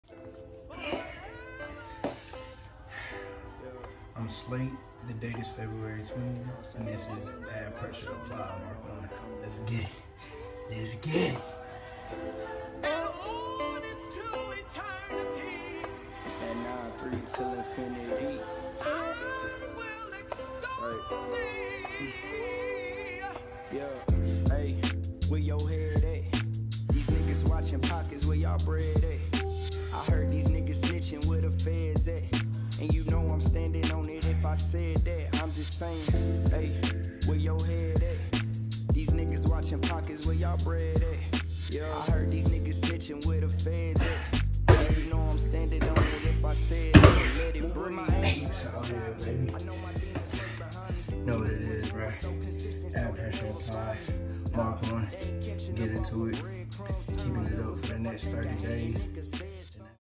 AB PRESSURE APPLIED MK I (DAY 1) | SlaterMcFly Fitness Shorts
AB PRESSURE APPLIED DAY I

Join me as I embark on a 6 month journey in which I aspire to sculpt my abs to their highest potential along w. making major improvements to my core strength. This journey will play a major role in my goal to pursue a spot in an upcoming season of American Ninja warrior. SlaterMcflyFitness.